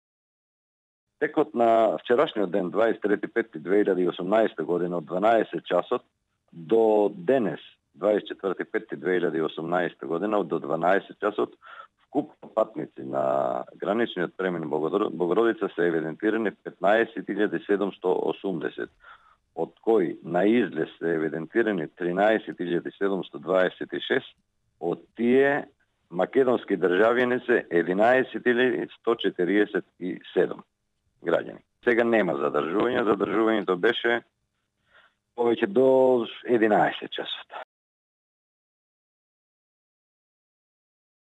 Тошев: Илјадници граѓани преминаа преку ГП „Богородица“ за влез во Грција
И овој продолжен викенд  нашите граѓани го користат за одмор во Грција.